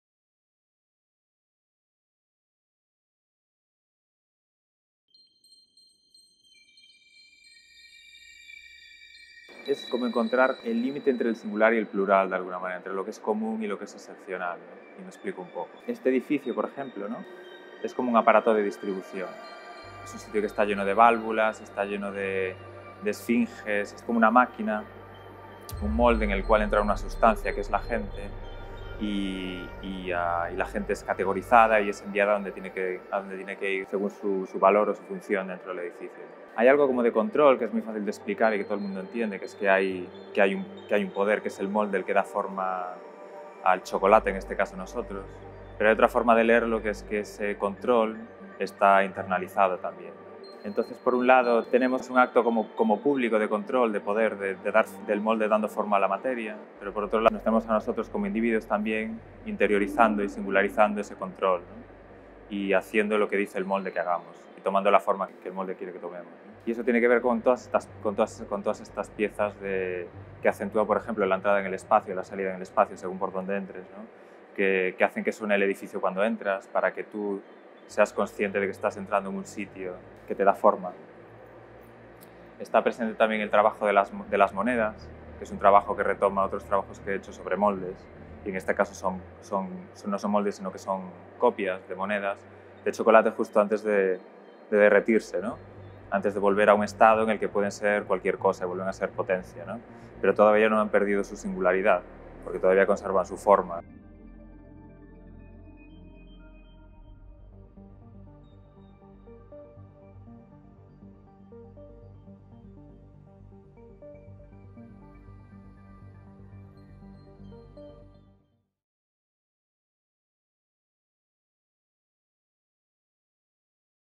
Exposición "Indigo Eye"
Del 1 de febrero al 24 de marzo de.2019.

La exposición “Indigo Eye” presenta la continuación de la investigación de Rubén Grilo en torno al chocolate planteando una reflexión sobre la asimilación (y a veces confusión) que se da entre placer y trabajo en la sociedad actual, así como la relación que se da entre tránsito y control, humano y material, o la puesta en duda del carácter único e irrepetible del molde que configura la identidad. La  muestra forma parte del ciclo "Mirror Becomes a Razor When It's Broken" comisariado por Sonia Fernández Pan.